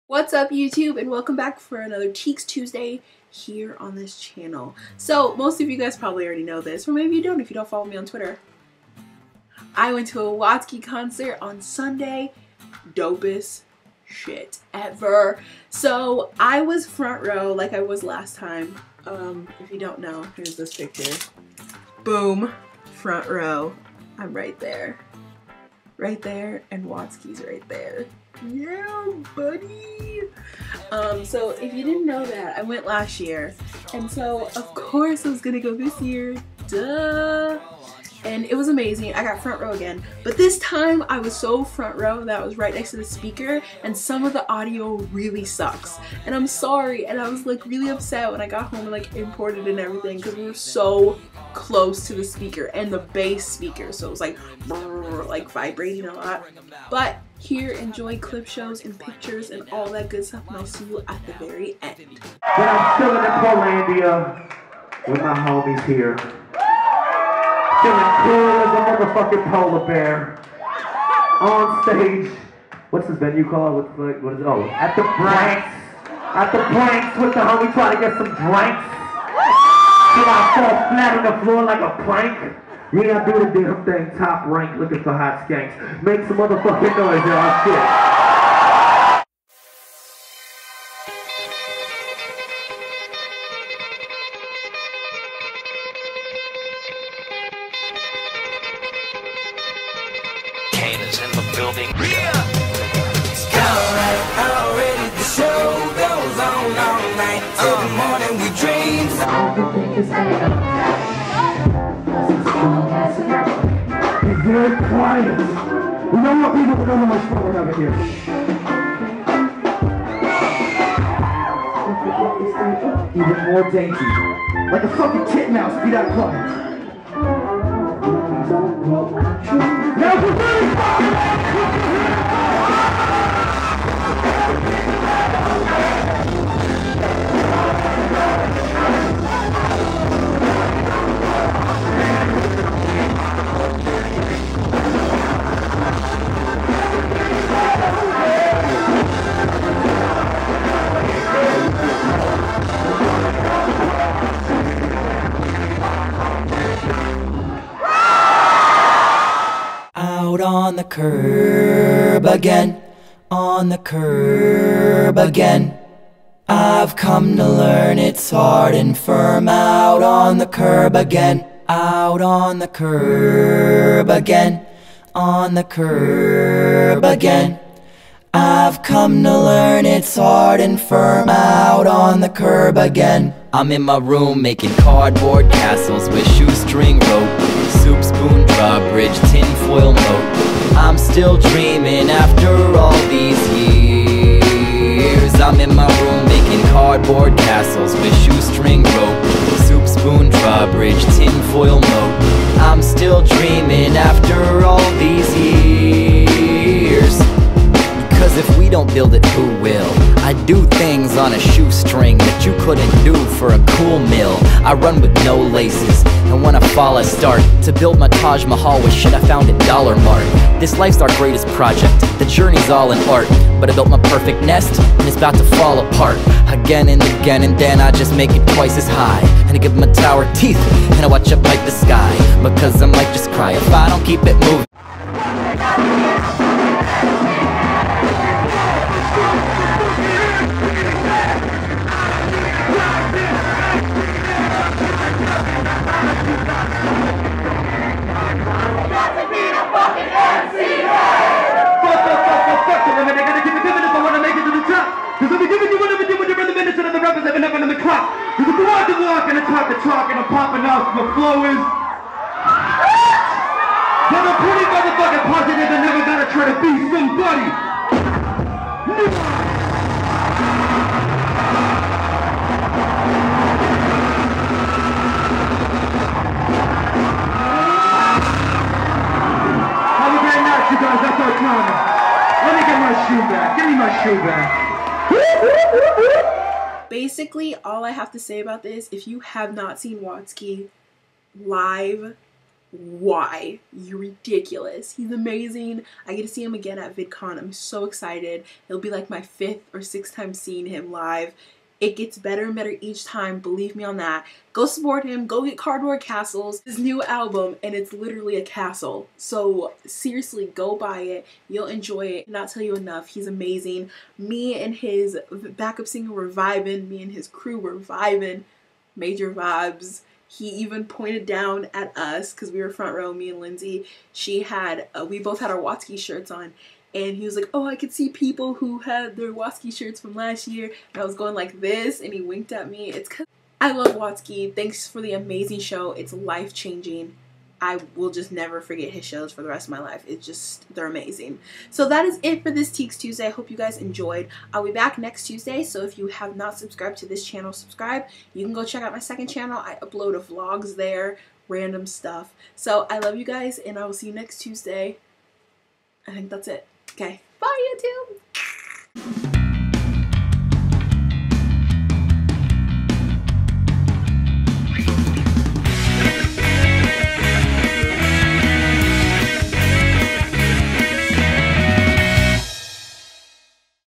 WATSKY FAM!!!
OPEN HERE

I LOVE watsky and this concert was AMAZING!!!
In the comments tell me your favorite concert EVER!